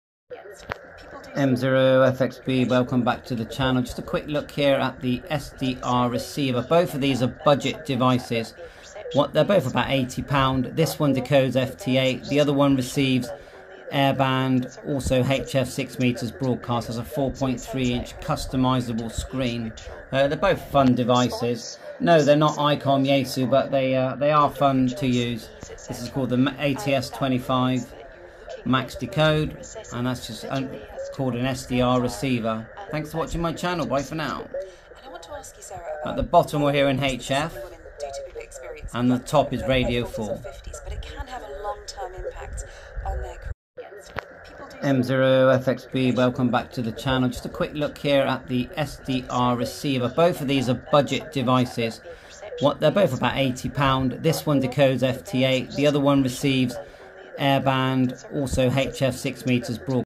ATS25 & Budget SDR 1.0 Radio Receiver 4.3 inch Waterfall Airband , HF 6 Meters ,VHF WFM
ATS 25
SDR BUDGET